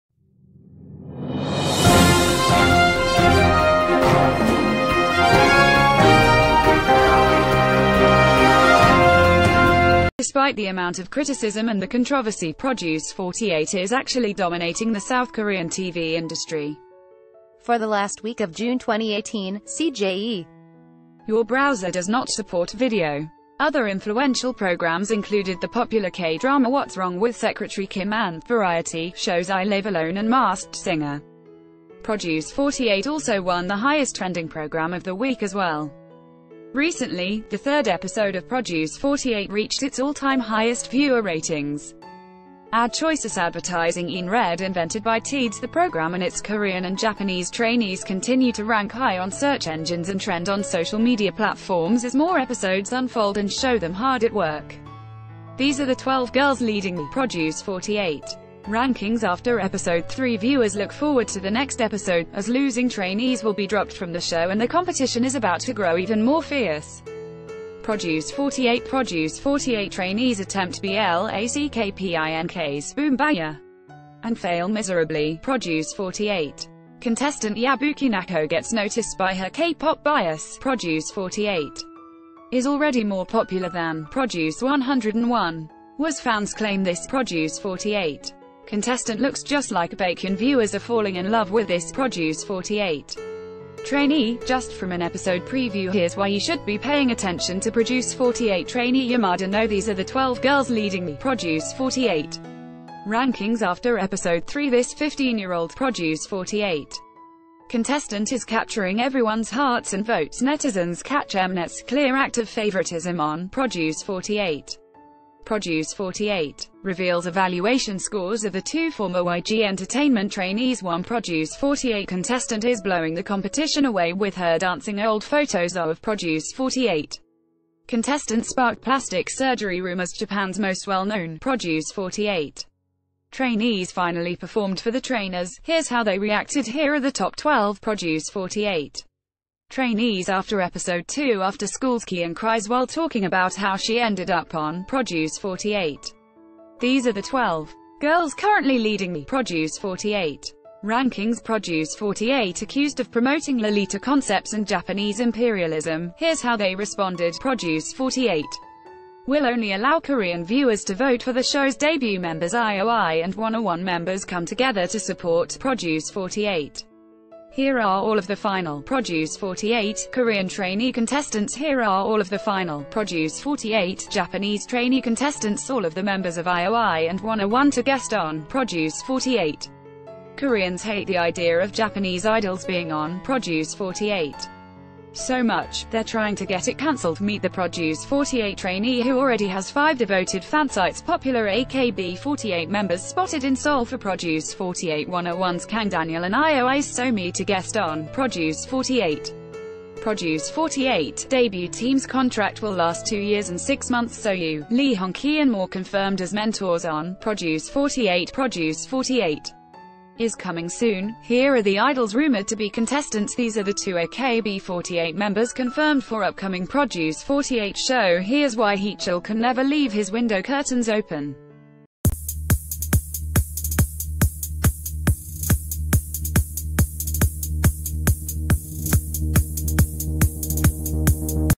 Produce 48 Named South Korea’s Most Influential TV Program
Despite the amount of criticism and the controversy, Produce 48 is actually dominating the South Kor...